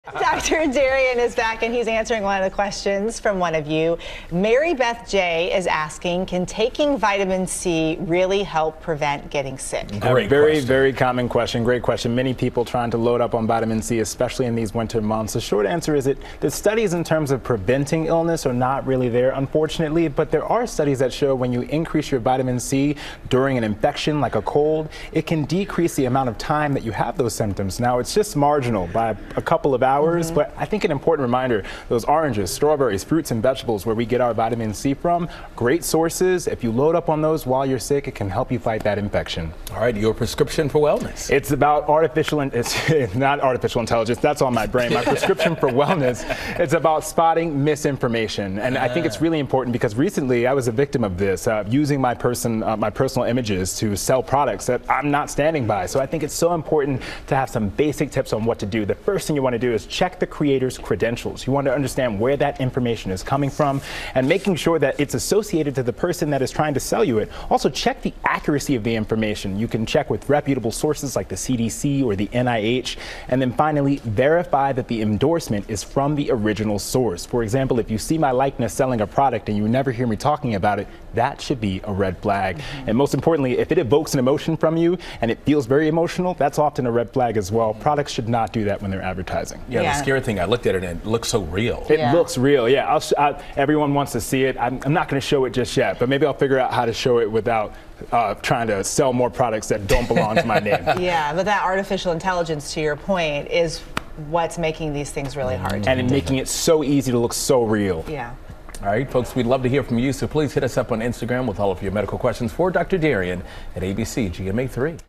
Can taking vitamin C prevent you from getting sick?
ABC News medical correspondent Dr. Darien Sutton answers your health questions and shares his daily prescription for wellness.